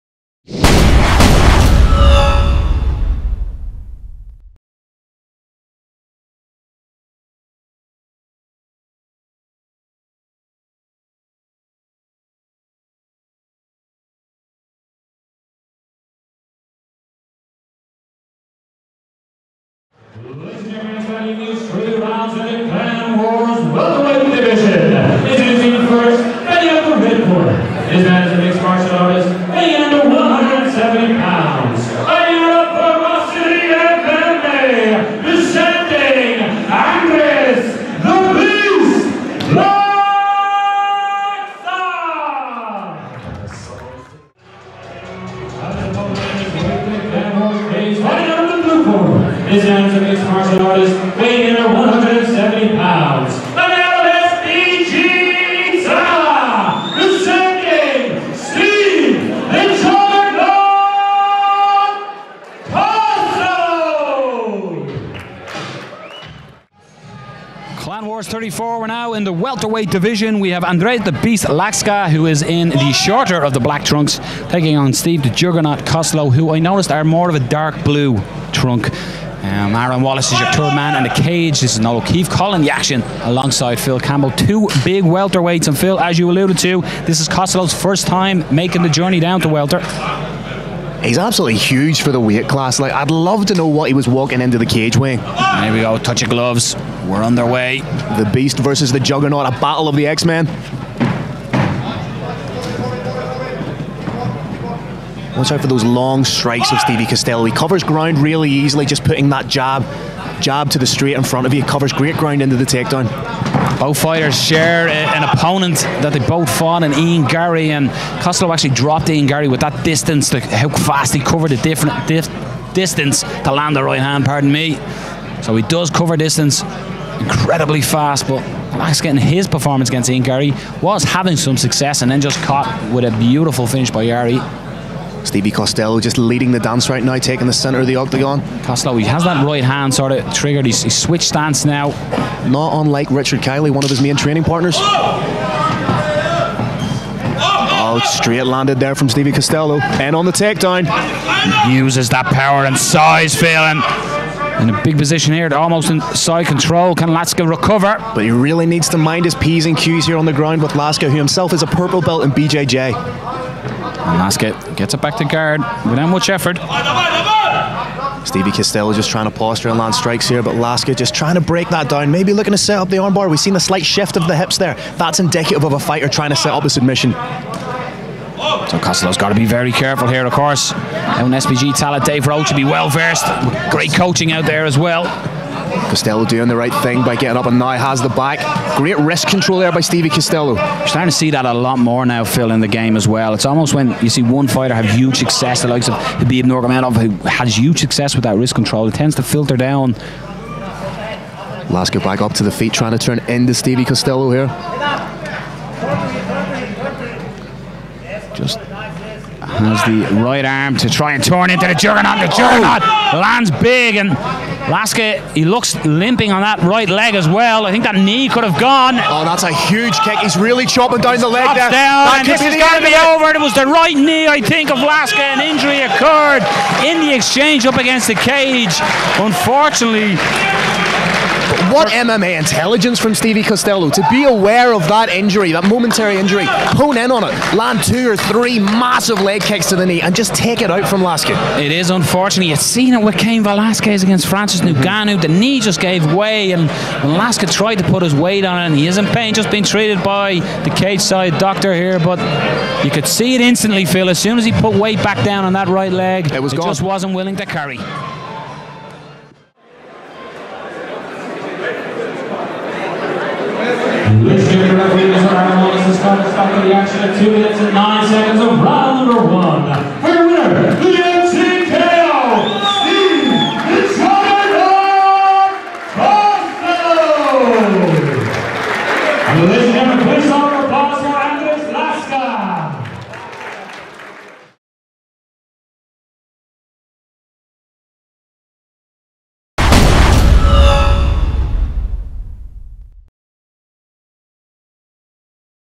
Andrejs Laksa vs Steve Costello - Clan Wars 34
Andrejs Laksa (Ferocity MMA) vs Steve Costello (SBG Tallaght)
AM Welterweight Bout - 170lbs - 3x3min

Clan Wars 34
The Crowne Plaza, Belfast
March 2nd 2019